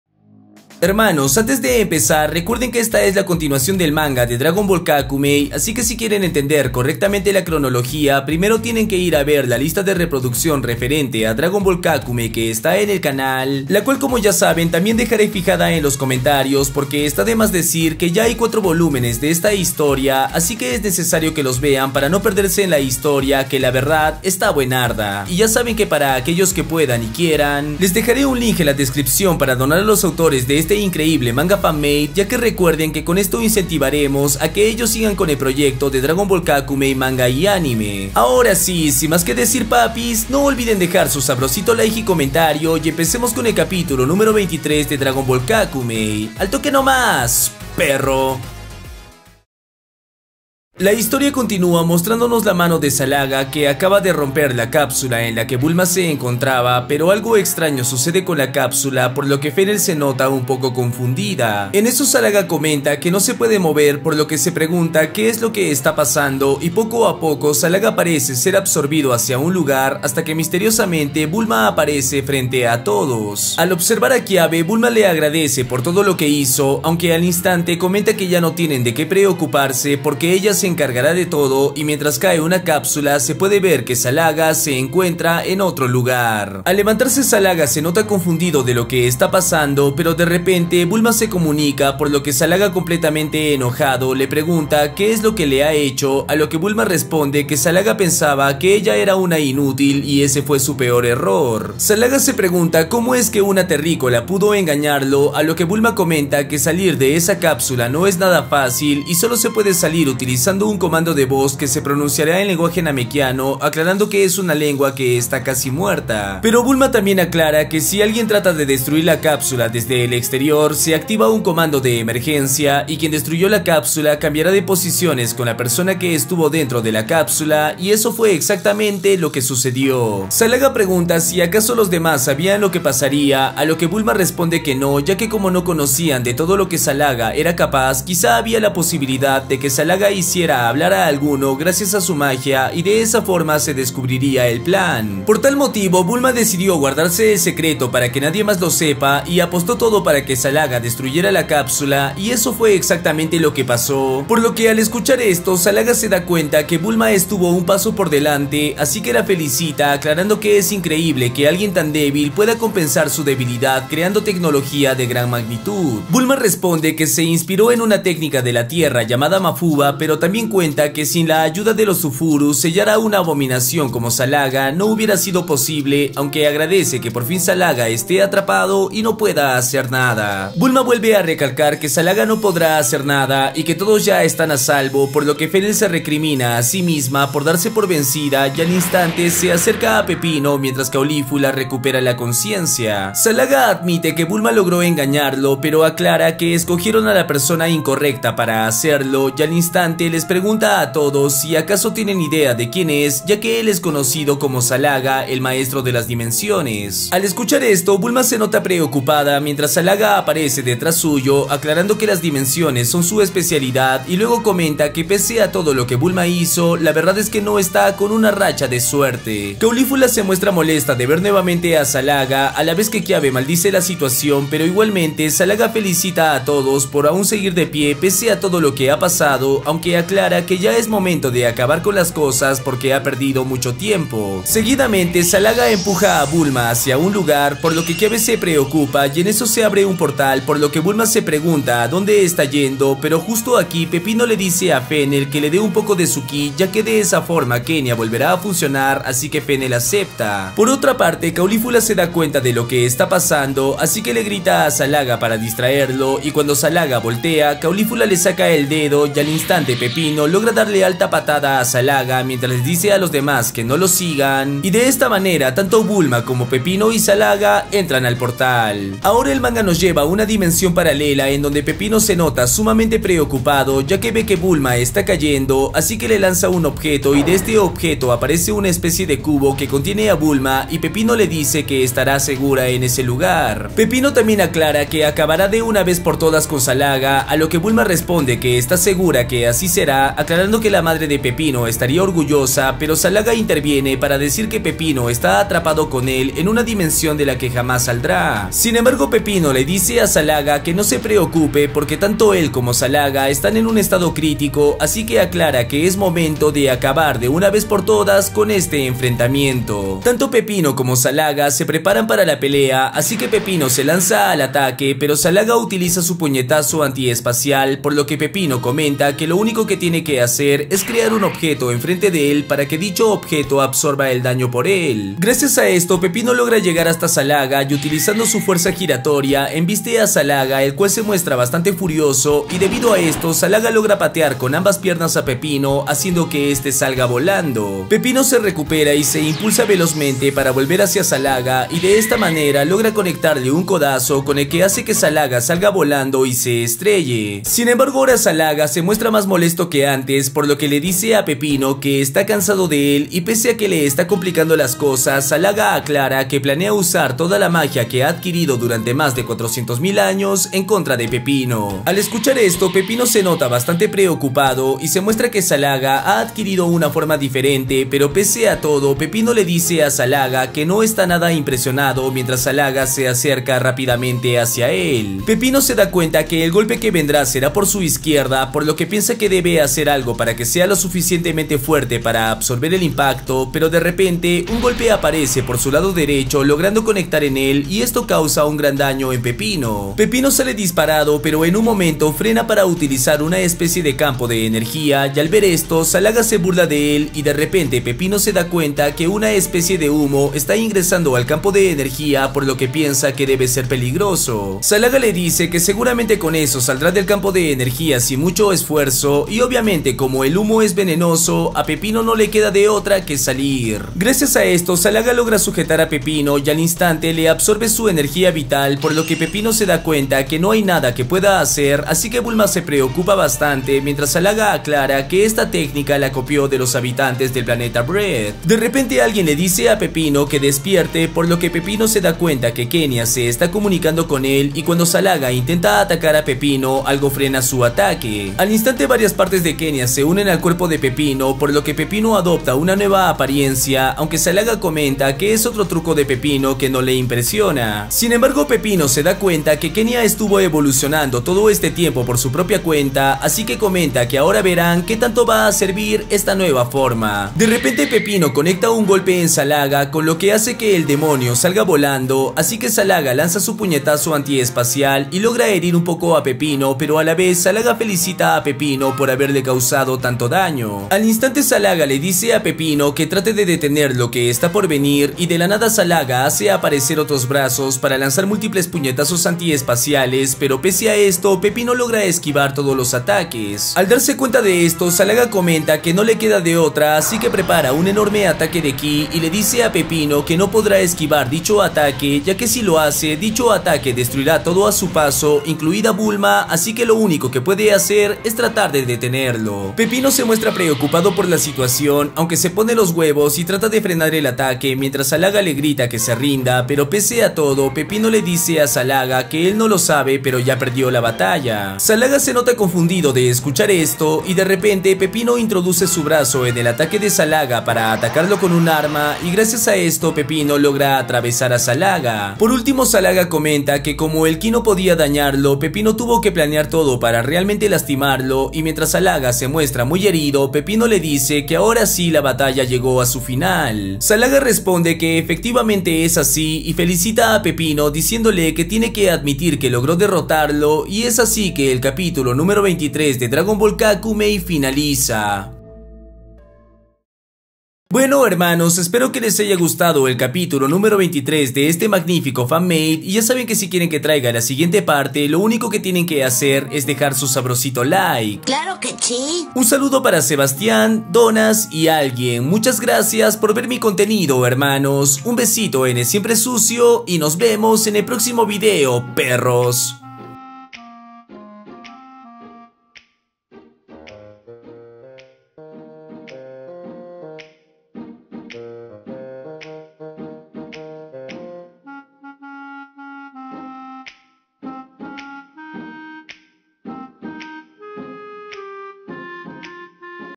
SALAGA USA SU ÚLTIMA TRANSFORMACIÓN CONTRA PEPINO - DRAGON BALL KAKUMEI 23 | DRAGON BALL SUPER
Marcas de tiempo:

0:00 introducción
0:50 Capítulo 23 de Dragon Ball Kakumei
9:07 Finalización

Otros títulos:

Explicación: Capítulo 23 de dragon ball kakumei
Análisis: Capítulo 23 de dragon ball kakumei
Resumen: Capítulo 23 de dragon ball kakumei